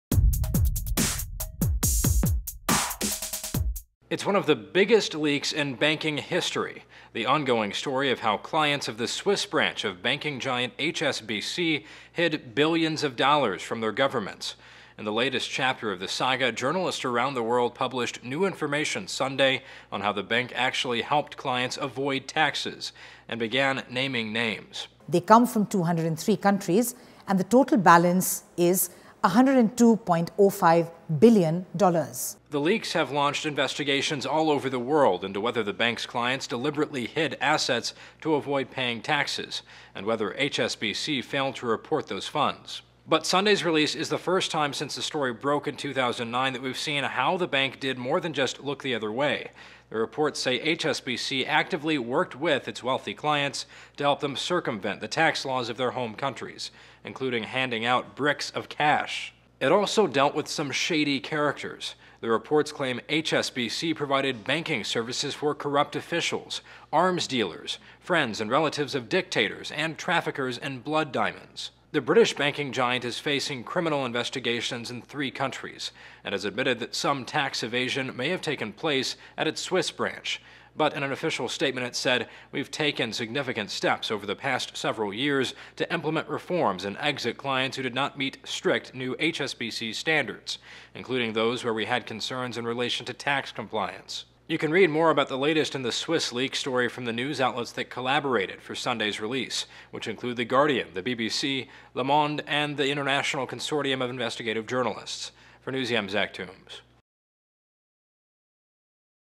New Reports Show HSBC Helped Clients Dodge Taxes
Data leaked in 2009 shows the Swiss branch of HSBC helped wealthy clients avoid paying taxes on their Swiss accounts.